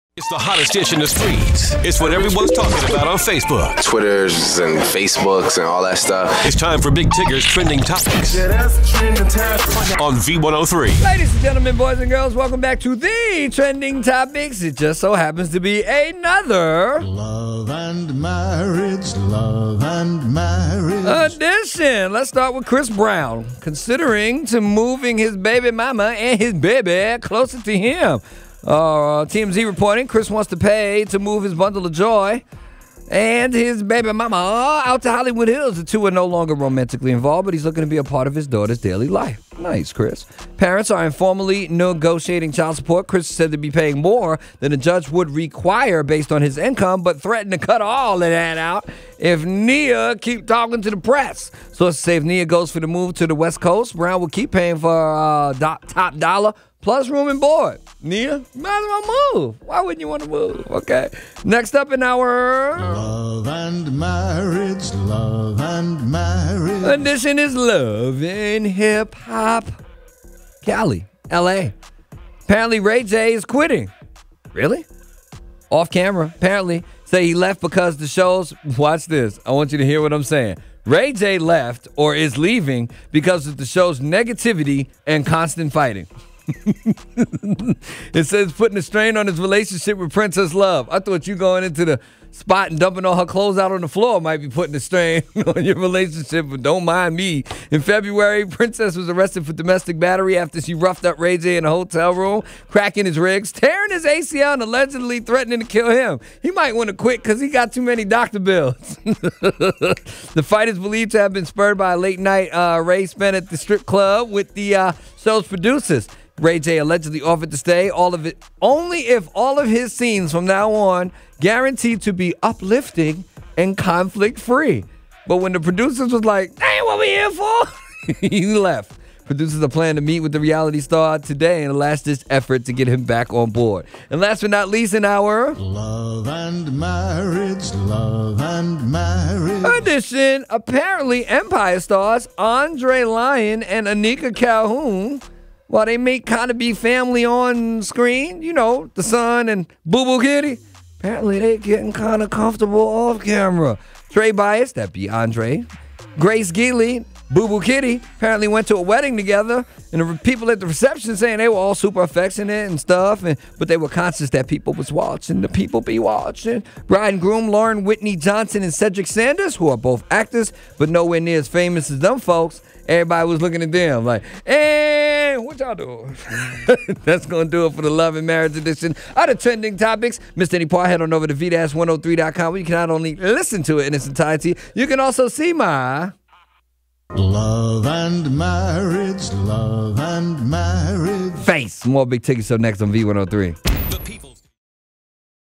Regarding 'LHHLA', Ray-J Hits The Ground Running First - Trending Topics
Chris Brown is willing to do a lot to be a good father.

Ray-J is willing to quit LHHLA to get peace of mind.

Two Empire stars are willing to give people something to talk about even off-camera.